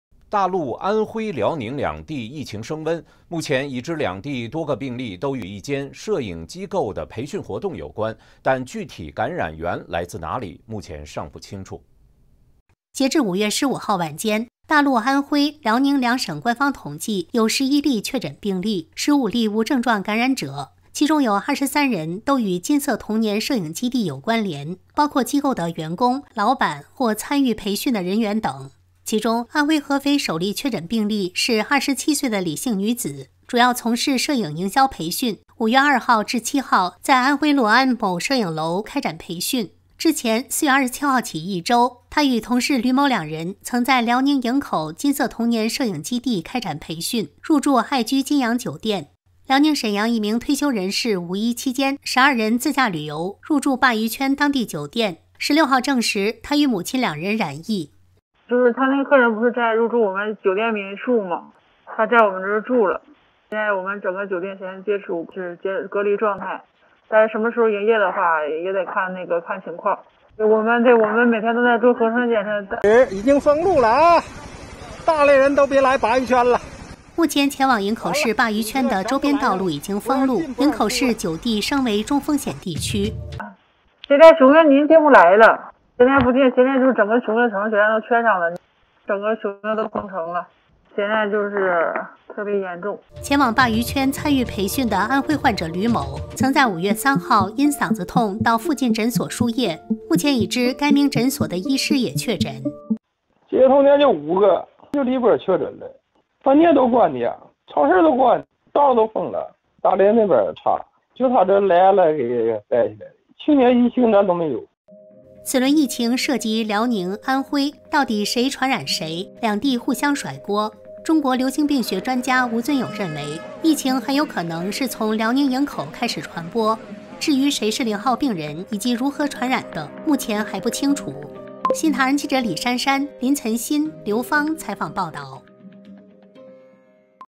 安徽遼寧疫情升溫 病源不清甩鍋急
大陸安徽、遼寧兩地疫情升溫。目前已知兩地多個病例都與一間攝影機構的培訓活動有關。但具體感染源來自哪裡，目前尚不清楚。